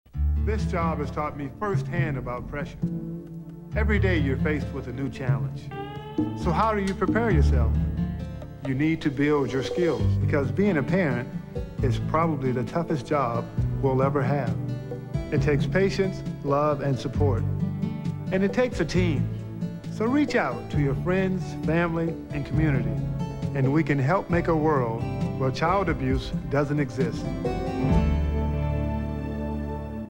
Clyde Drexler Talks Parenting & Child Abuse (1996)
Clyde Drexler was a 10-time all-star who won an NBA championship as a member of the Houston Rockets in 1995. Since this is a commercial about parenting, it seems notable to mention that he has four children, Erica, Austin, Elise, and Adam.